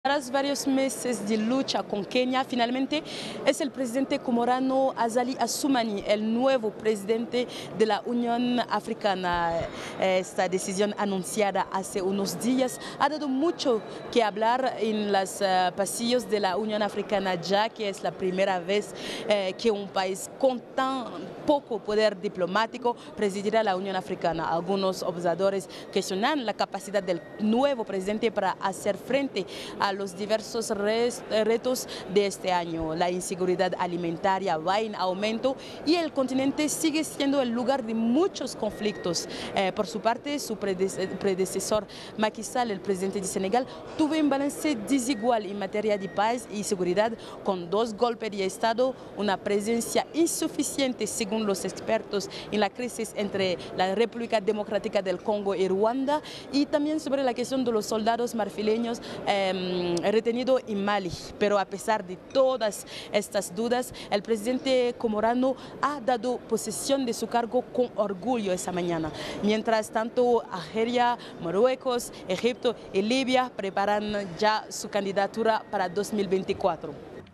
Informe desde Adís Abeba: crisis de seguridad, uno de los temas base de cumbre de Unión Africana
Los conflictos armados desde el Sahel en África Occidental hasta el Cuerno de África en el Este y los efectos de las sequías y las inundaciones han expulsado a cada vez más africanos de sus hogares, con un número de desplazados al sur del desierto del Sáhara que ha aumentado más del 15% en el último año, según cifras de Naciones Unidas. Informa Miriam Koné.